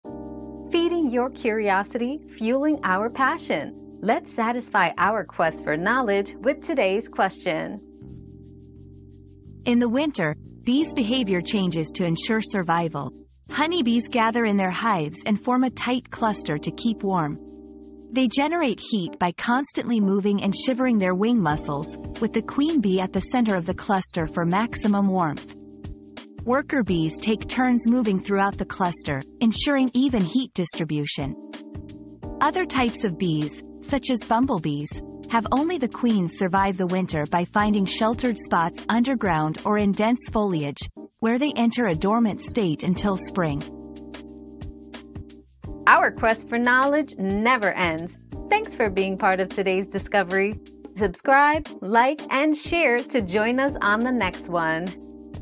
Where do bees go in the winter?
Survival Secrets: Where Bees Go in Winter 👉 Bee Winter Survival 👉 Discover how bees survive the cold winter months by huddling together in their hives or finding sheltered spots underground. Queen bees take charge to ensure the colony's survival until spring comes.

Laura S. Harris (2024, April 28.) Where do bees go in the winter?
    🌐 AskAbout.video